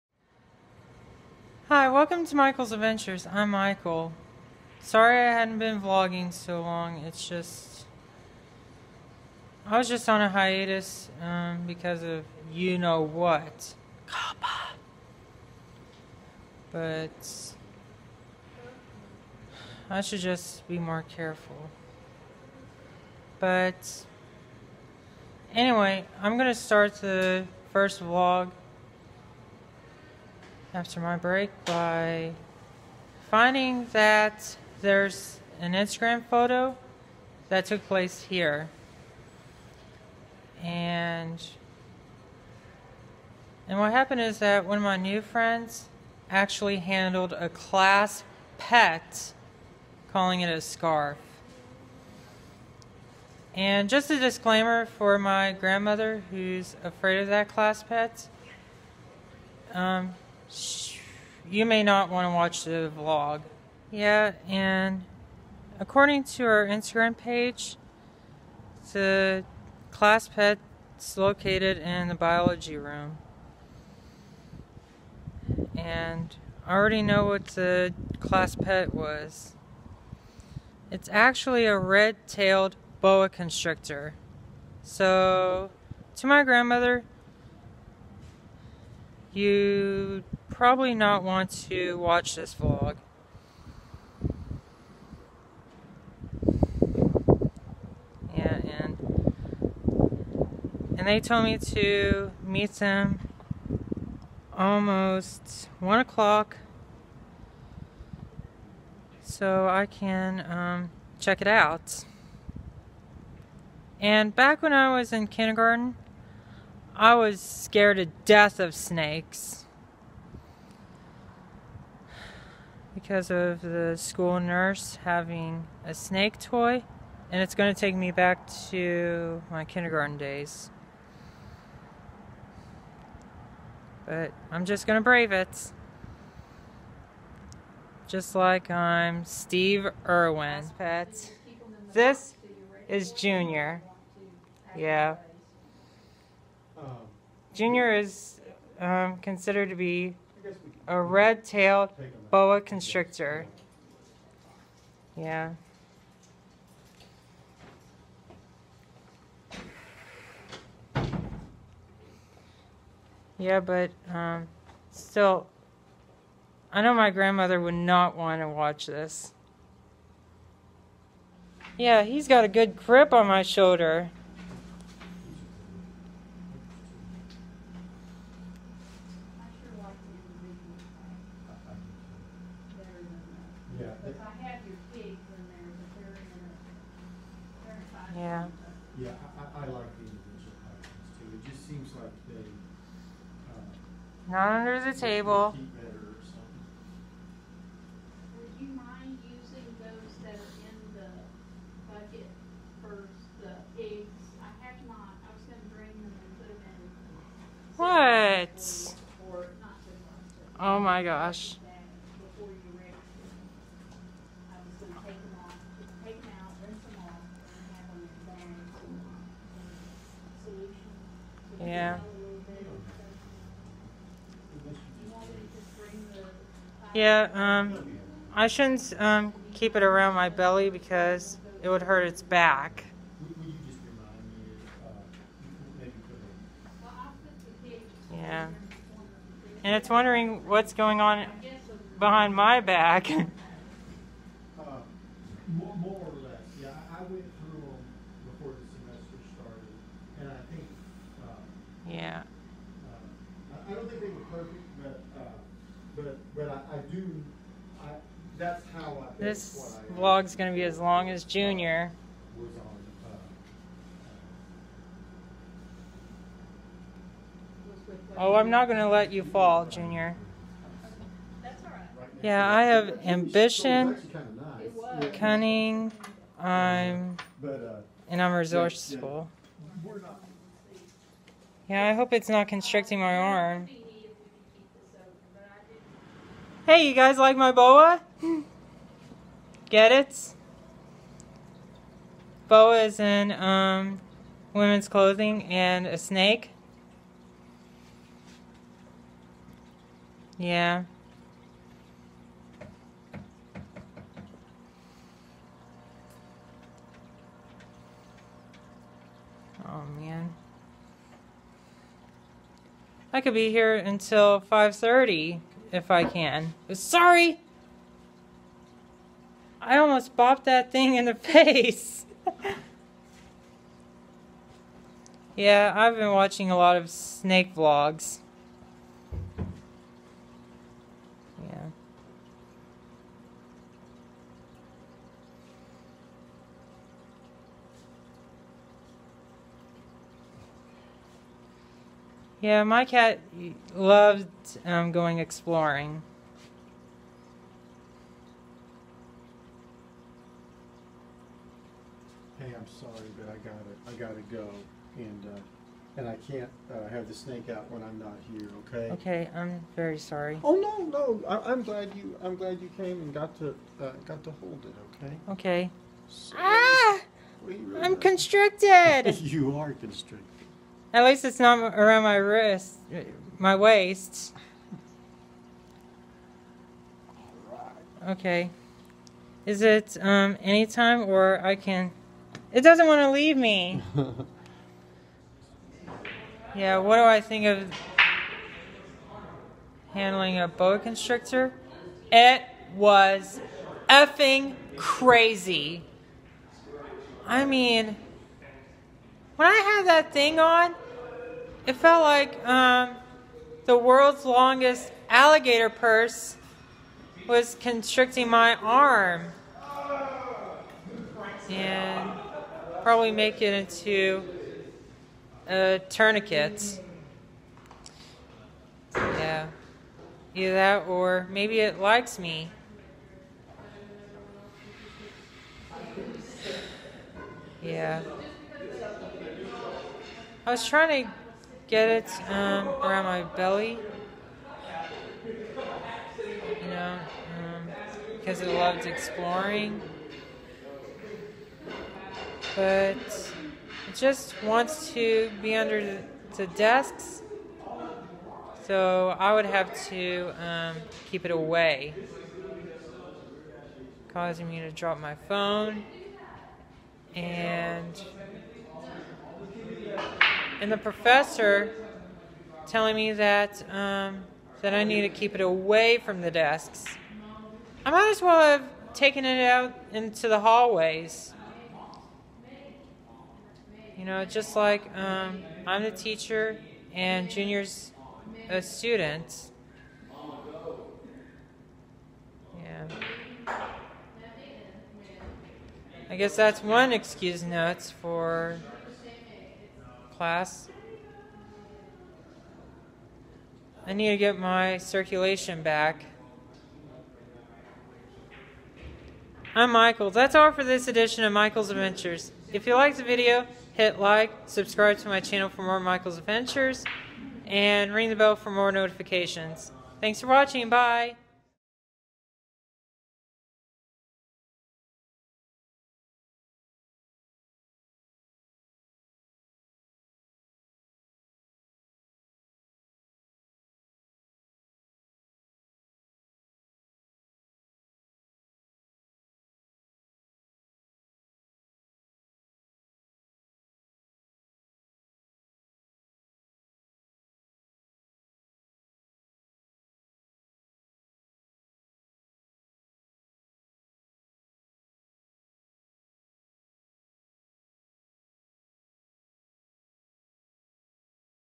HOLDING A BOA CONSTRICTOR (INSANE)!!! | MichaelBlane Walker | February 4th 2020
NOT FOR PEOPLE WHO ARE AFRAID OF SNAKES!!!

Kicking off a new year, I held a boa constrictor to conquer my childhood fear. I was afraid of snakes when I went by the nurse’s office and she had a toy snake that hissed at me.
Now, I held one, which was insane!
Would you guys try it?

Thanks to Prof. Michael Keck for being with me.